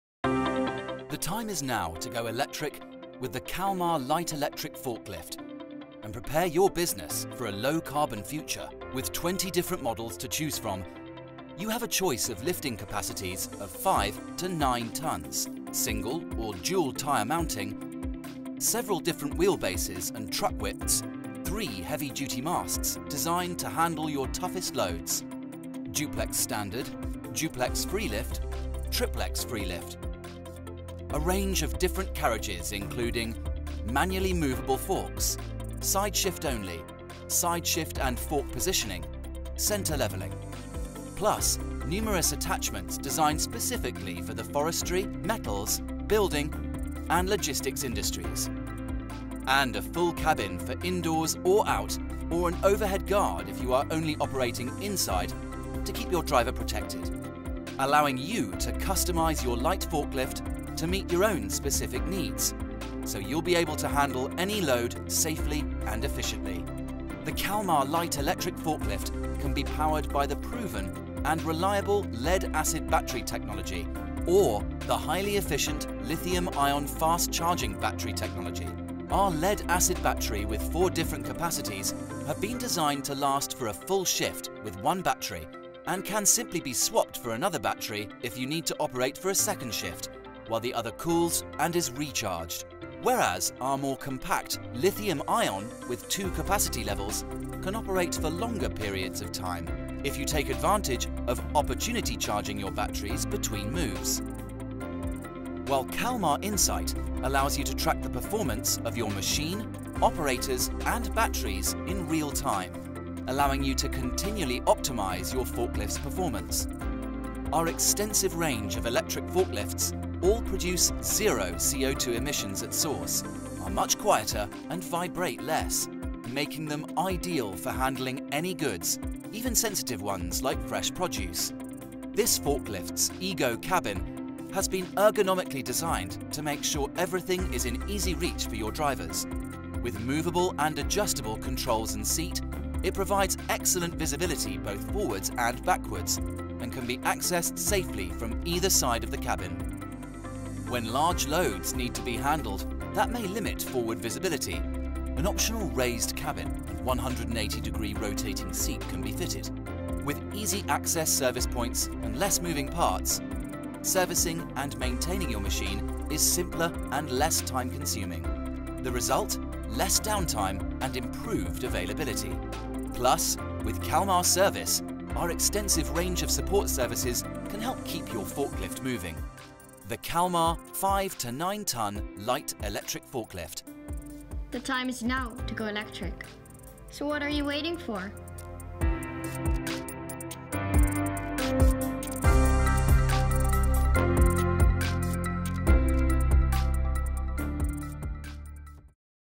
Electric Forklift Truck for 5-9 tons | Kalmar electric lift trucks
Kalmar Electric Light Forklift Truck represents the latest generation of electric forklift trucks, offering exceptional performance without the emissions, noise, and vibration typically associated with diesel trucks. With a focus on operational efficiency, low maintenance costs, and extended service life, it proves to be a cost-effective investment. Learn more about Kalmar Electric Light Forklift Truck

Kalmar Electric Light Forklift Truck is equipped with advanced features such as an efficient battery usage system, industry-leading diagnostics, and a customizable EGO cabin for optimal operator comfort, the forklift ensures a pleasant driving and maintenance experience. The energy-efficient technology, including the option for Eco mode, guarantees a smooth and quiet operation, making it suitable for a range of indoor and outdoor lifting operations. With a strong emphasis on safety, environmental sustainability, and battery protection, the Kalmar Electric Light Forklift Truck stands out as a versatile and reliable solution for businesses looking to optimize their fleet performance.